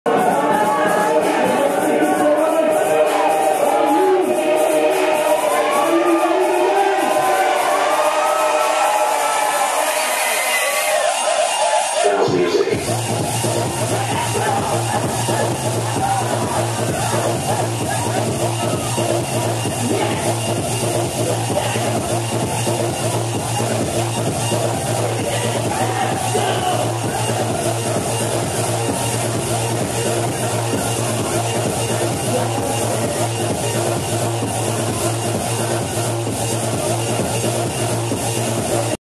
DJ Dheeru Beatz Live @ Government Engineering College Idukki (Advaya 4.0 fest)
DJ Dheeru Beatz playing Live @ Government Engineering College Idukki for Advaya 4.0